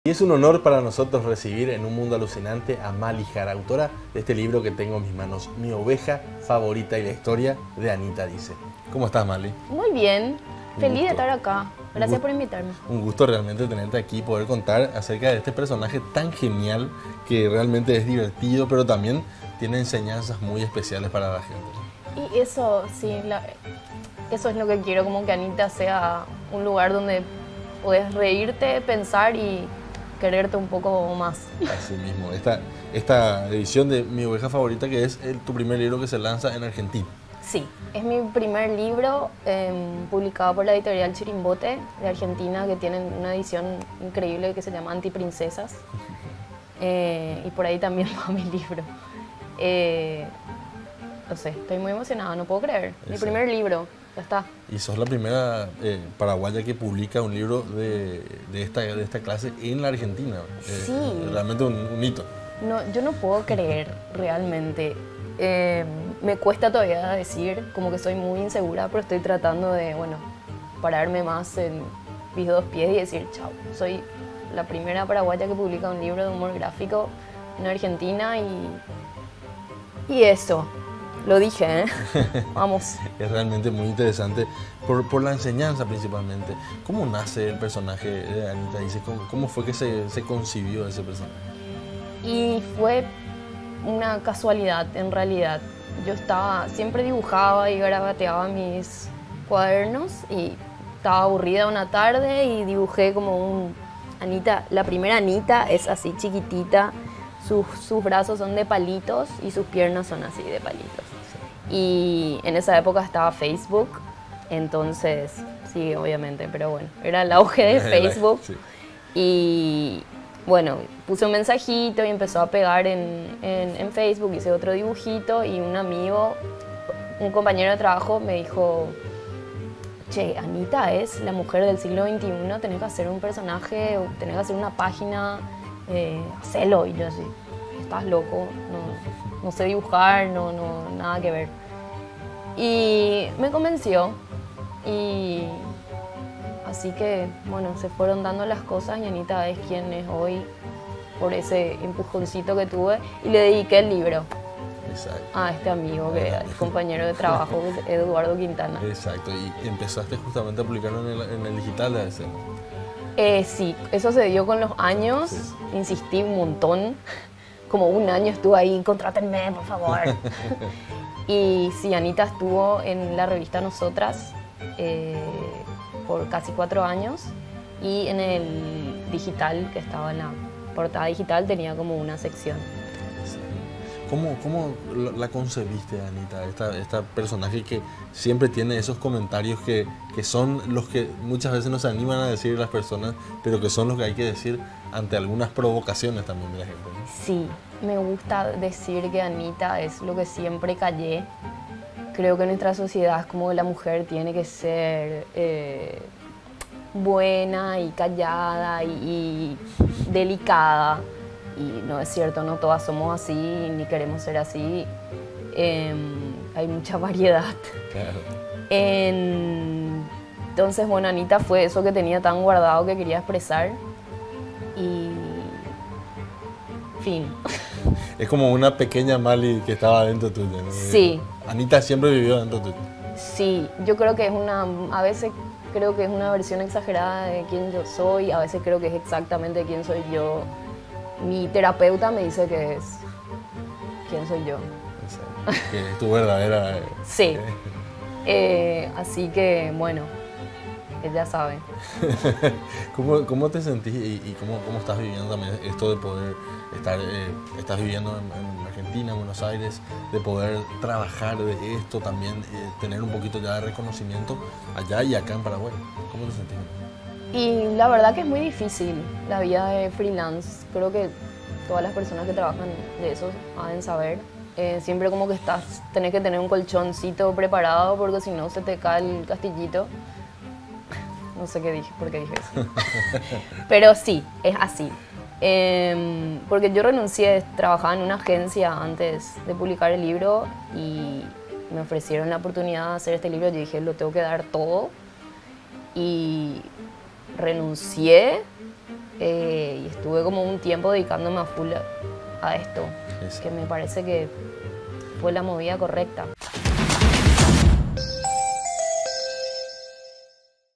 ‘’Mi oveja favorita’', una historia de Anita Dice
“Mi oveja favorita”, el primer libro de Anita Dice . Es el primer material de humor gráfico publicado por una paraguaya en Buenos Aires, Argentina. Con el sello de la editorial argentina Chirimbote, Mi oveja favorita es el primer libro de humor gráfico publicado por una paraguaya en Buenos Aires, Argentina, donde Mali Jara vive hace ocho años.